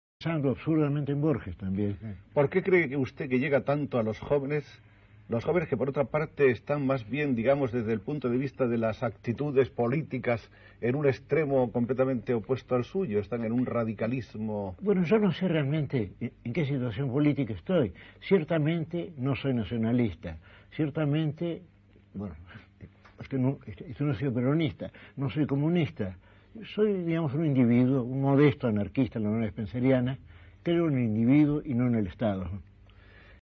Soy un modesto anarquista a la manera spenceriana | Jorge Luis Borges
"Bueno, yo no sé realmente en qué situación política estoy. Ciertamente no soy nacionalista, ciertamente no soy peronista, no soy comunista. Soy, digamos un individuo, un modesto anarquista a la manera spenceriana [en referencia a Herbert Spencer], creo en el individuo y no en el estado".

Fragmento de la entrevista que concedió Jorge Luis Borges a Joaquín Soler Serrano. España, 1976.